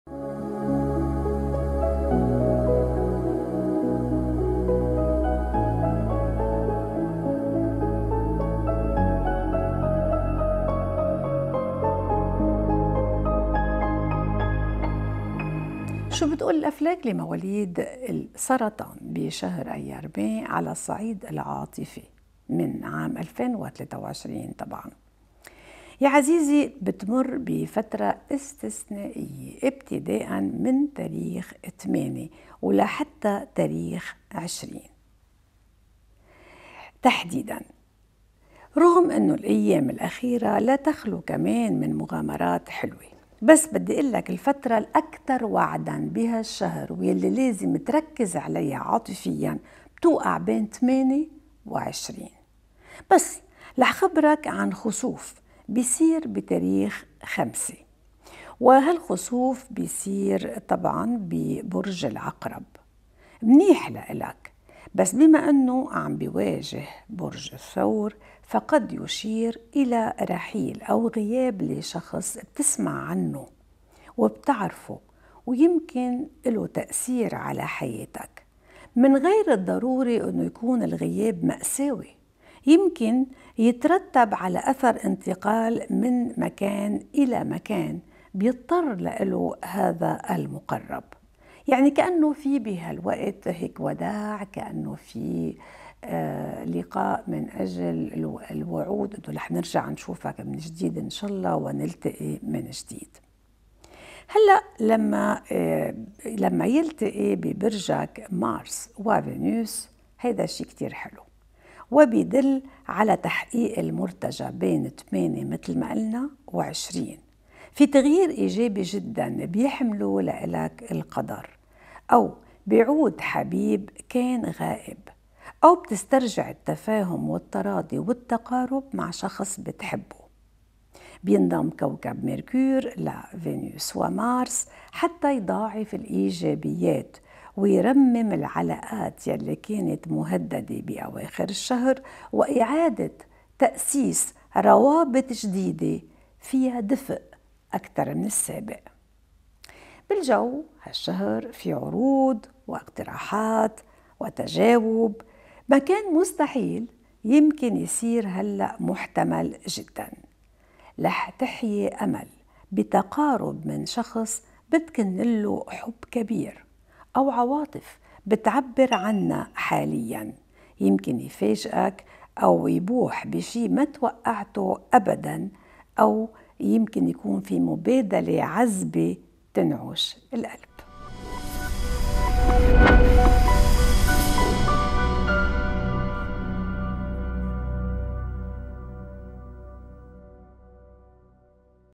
ماغي فرح - برج السرطان - الصعيد العاطفي لشهر ايار - مايو -٢٠٢٣ /Maguy Farah - Cancer -May 2023
24 تشرين الأوّل/ أكتوبر/ October - 21 تشرين الثاني/ نوفمبر/ November

مواليد الدائرة الأولى (من 24 تشرين الأوّل/ أكتوبر/ October إلى 2 تشرين الثاني/ نوفمبر/ November)
مواليد الدائرة الثانية (من 3 إلى 12 تشرين الثاني/ نوفمبر/ November)
مواليد الدائرة الثالثة (من 13 إلى 21 تشرين الثاني/ نوفمبر/ November)

شاهد التوقعات الكاملة لـ السرطان ٢٠٢٣  - Watch the Yearly Cancer Horoscope for 2023